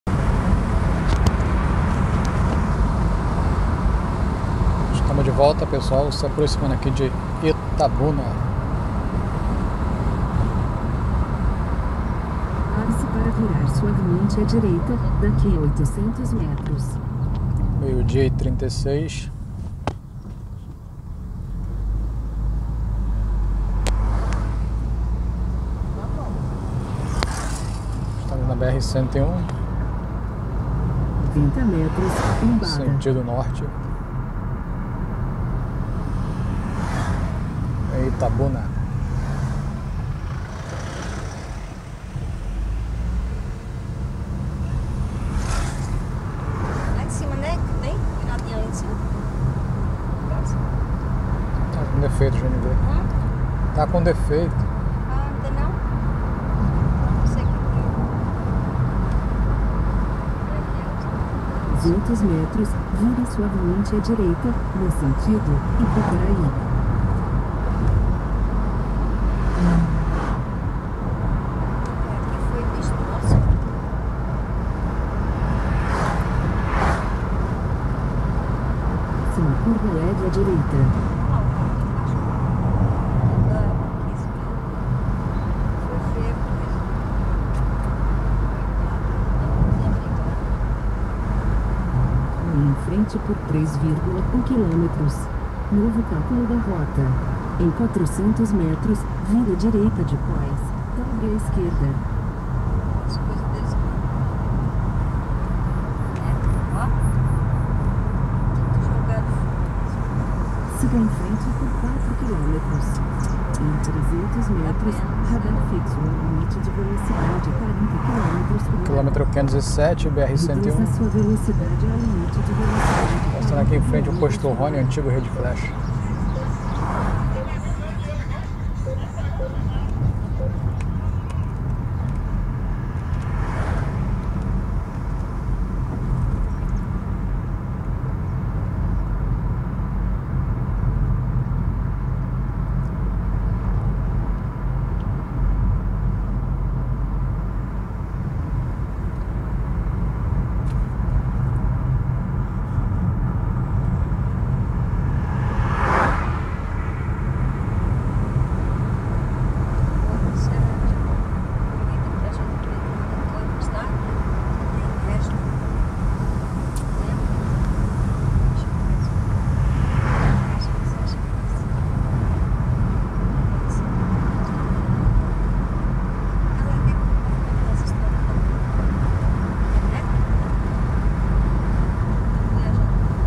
Viagem para Campina Grande PB vídeo 17 em Itabuna Bahia parte 01
Viagem do RJ para Campina Grande Paraíba passando em Itabuna Bahia